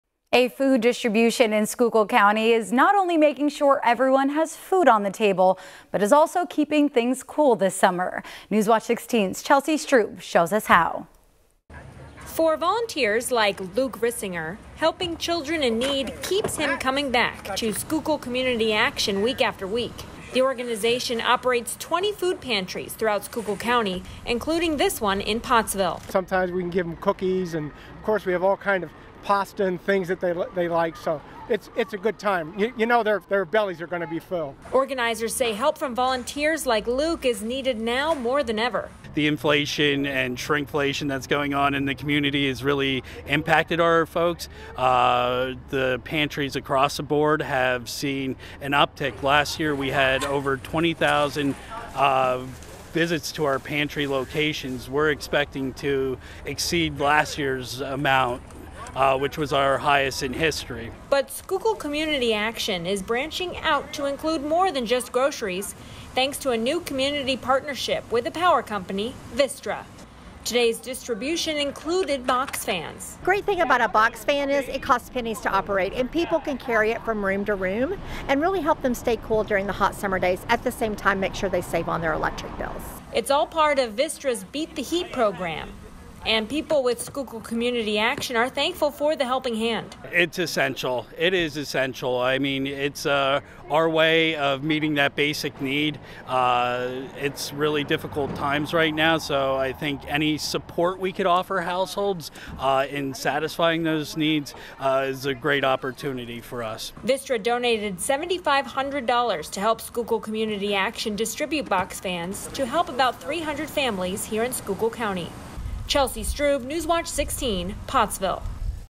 Feeding families and keeping them cool in Pottsville
A weekly food distribution in Schuylkill County came with something special this week to 'beat the heat'